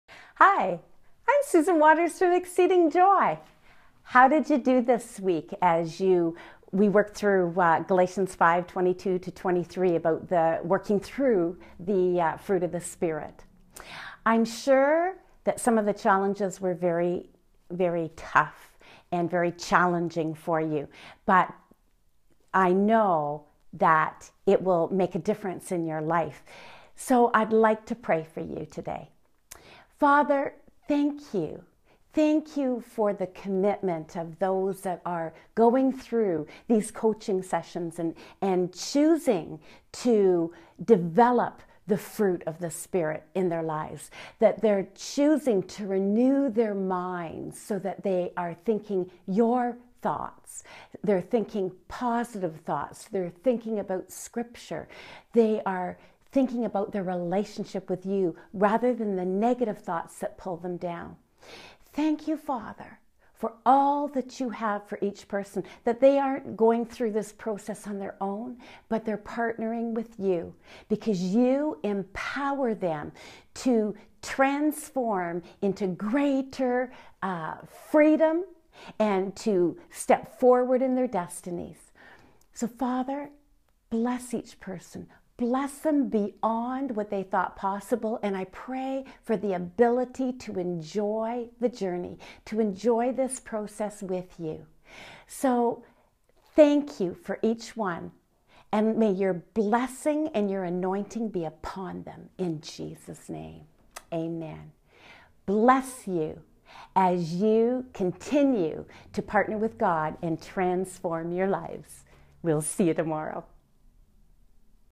Transformation Video # 4.7 "Prayer" by Susan Waters from
Transformation Video # 4.7 "Prayer" by Susan Waters is Susan's prayer for you this week.